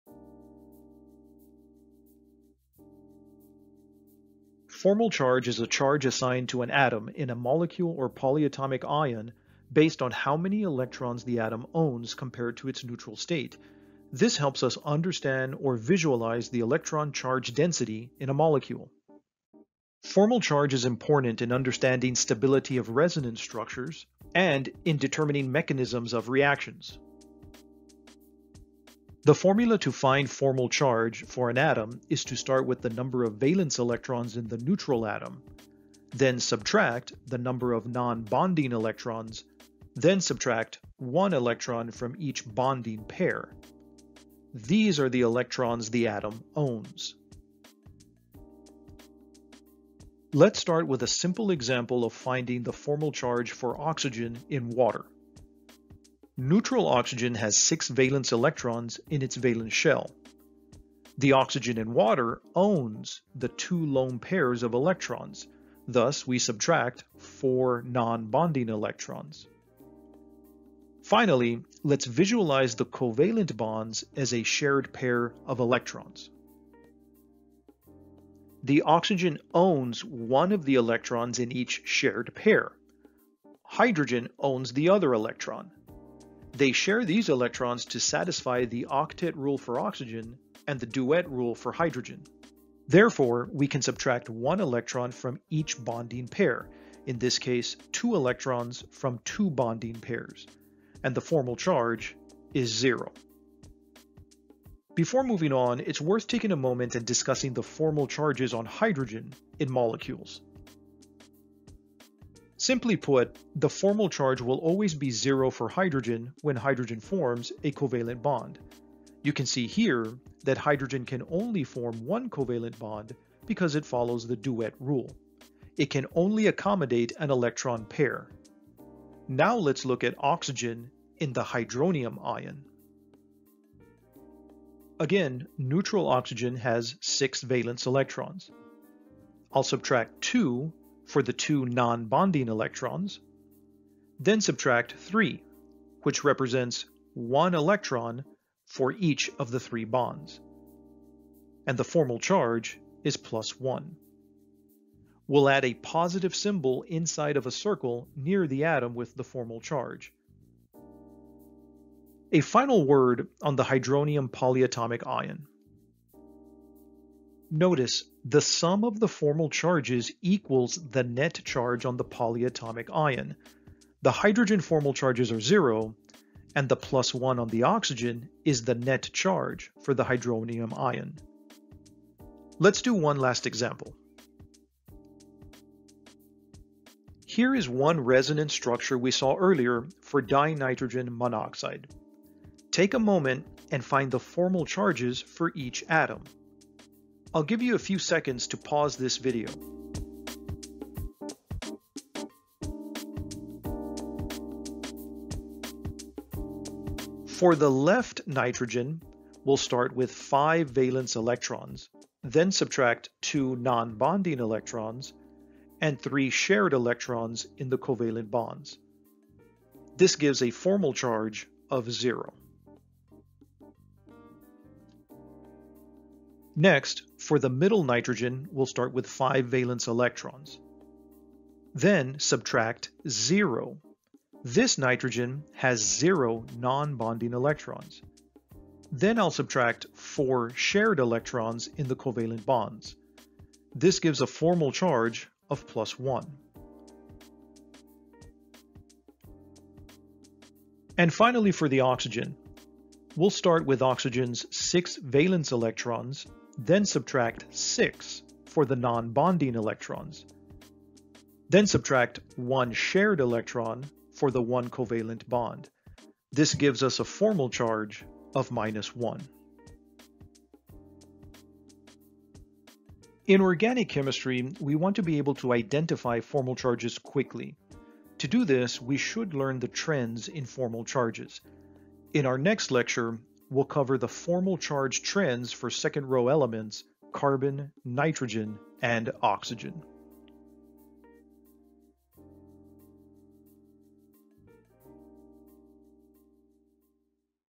Formal Charge in General Chemistry
The rules for calculating the formal charge of atoms in molecules and polyatomic ions is explained. Examples of finding the formal charge of oxygen in water and the hydronium ion are presented along with finding the formal charges of all the atoms in dinitrogen monoxide, N2O.
Video Thumbnail: Ah, yes, chemistry; so formal, so classy, just like the photo.
Video Thumbnail Photo Credit: MS365 Powerpoint
See video below for common trends in formal charge of carbon, nitrogen and oxygen; the most common elements in organic chemistry.